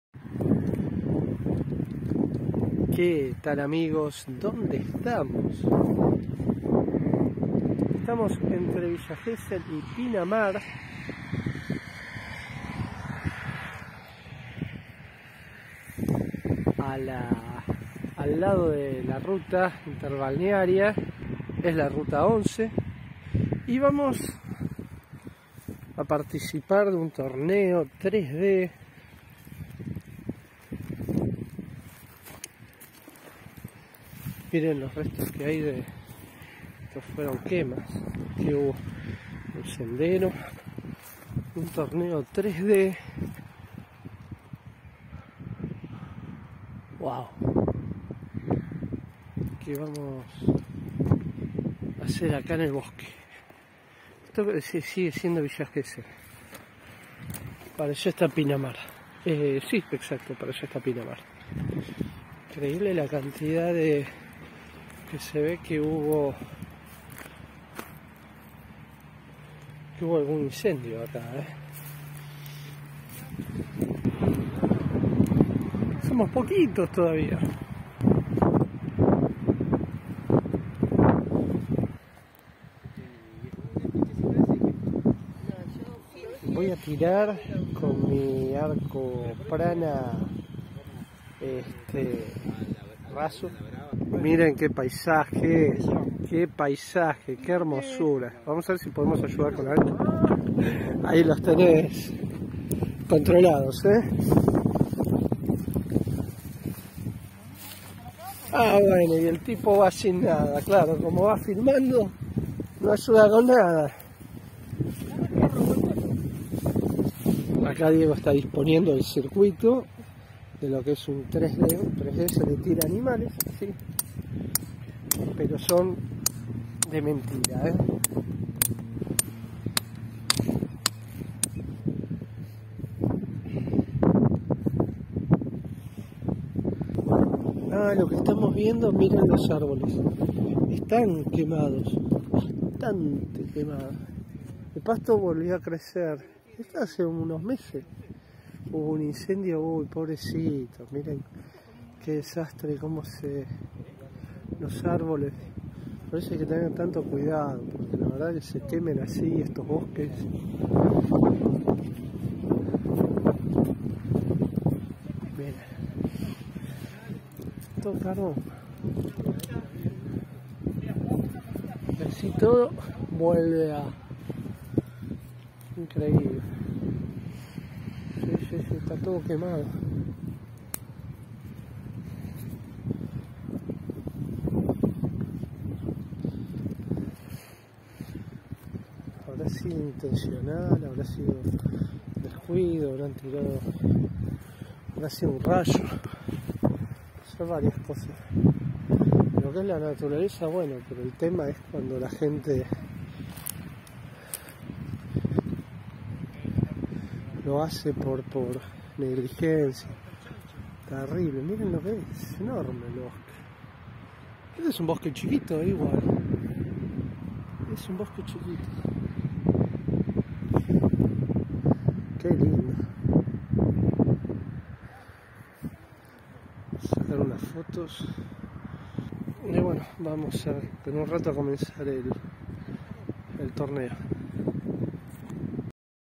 Canal JDB. Cómo es un Torneo 3 D de arquería en un Bosque. Pte 1 preparativos.
tenemos el gusto de participar en un mini torneo 3D de arquería organizado por la escuela 3D archer de Villa gesell. eres la primera parte muestro cómo es la ubicación cómo se organiza el circuito de tiro del torneo